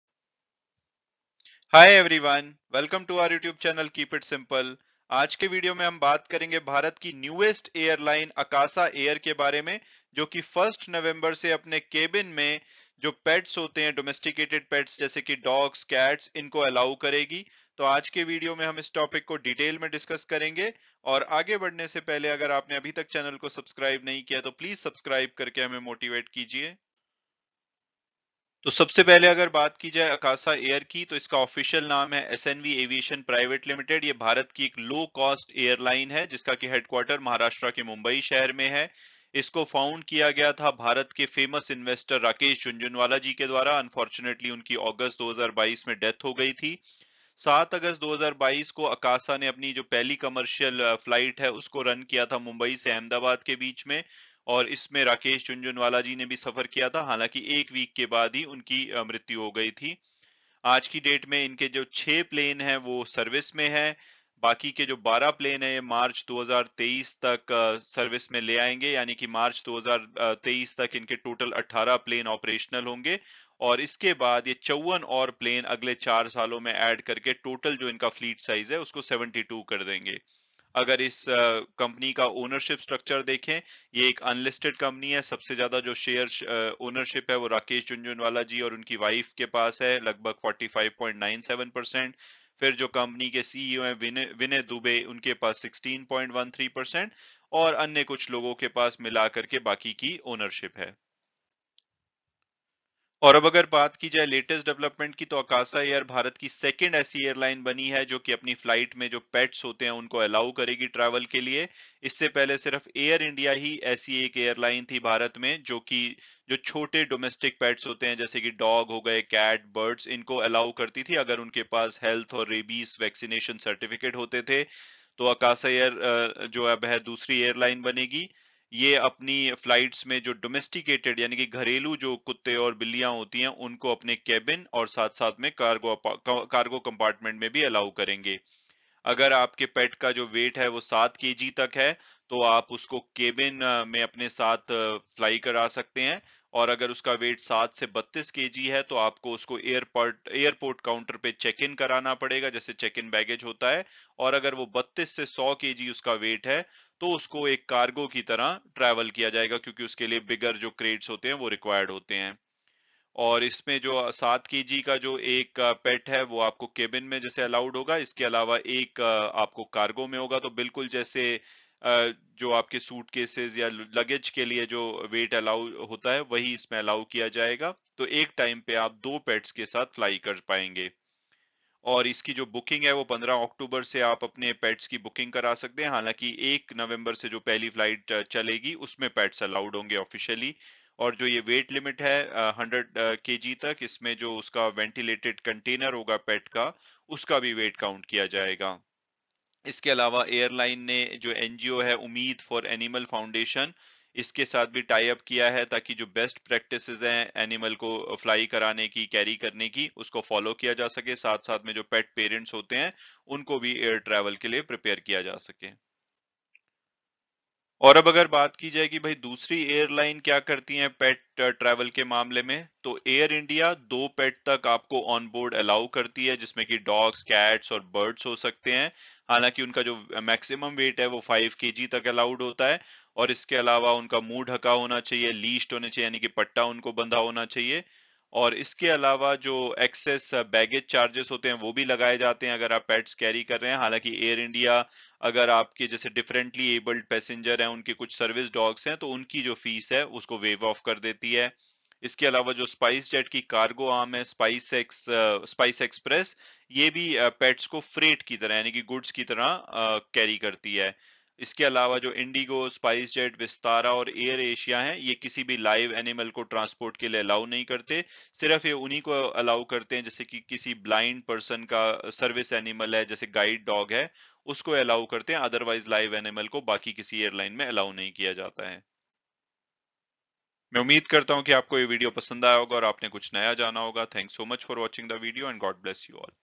Akasa Air to allow pets in cabin and cargo! Good News for Pet Lovers!!!
This video is in Hindi and covers the following topics:

Akasa Air, a brand of SNV Aviation Private Limited, is an Indian low-cost airline headquartered in Mumbai, Maharashtra, India

It was founded by Indian billionaire Rakesh Jhunjhunwala (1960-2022).

The airline began commercial operation with its first flight service from Mumbai to Ahmedabad on 7 August 2022, after receiving its first Boeing 737 MAX aircraft

Akasa Air's fleet size will be 18 aircraft by the end of March 2023 and over the next four years, the airline will add 54 additional aircraft, taking its total fleet size to 72 aircraft

Akasa Air has become the second airline company to allow pets on flights.

Till now, only Air India allowed "small inoffensive domestic pets" like dogs, cats and birds, accompanied by Health and Rabies vaccination certificates, to fly.

The airline will allow “domesticated dogs and cats” in passenger cabin as well as cargo compartment.

If your pet weighs up to 7kg, it can travel in the cabin onboard.

But if your pet weighs anywhere between 7kg to 32 kg, they will need to be checked in at the airport counters, and those weighing between 32-100kg will need to be checked in as cargo as they will need bigger crates to be flown in.

Only one pet weighing up to 7kg will be allowed in the cabin and one in the cargo hold, which means one passenger will be allowed two pets on a flight.

The airline will commence bookings for pets from October 15 for flights from November 1 onwards.

The weight limit includes the weight of the ventilated container used for carrying them.

The airline has also collaborated with NGO Umeed for Animals Foundation for incorporate industry best practices for carriage of animals as well as help pet parents as well as pets to prepare for air travel.

Air India allows up to two pets onboard.

These include dogs, cats and birds.

However, they cannot be heavier than five kg.

Pets have to be muzzled and leashed.

Airlines often also levy excess baggage charges for carrying pets, however some like Air India waive the fee for service dogs used by differently abled passengers.

SpiceJet’s cargo arm called SpiceXpress also carries pets as freight.

IndiGo, SpiceJet, Vistara and AirAsia do not allow the transport of any live animal, except for service animals such as a guide dog for a blind person.